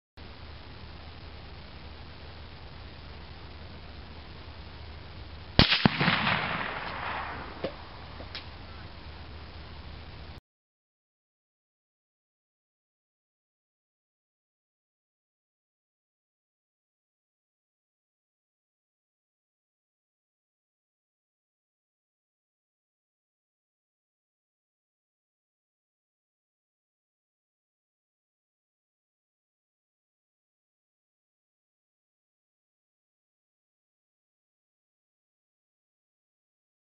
Exploding Paint Can Slow Motion
Shot a paint can at 200 yards with a .223 rifle.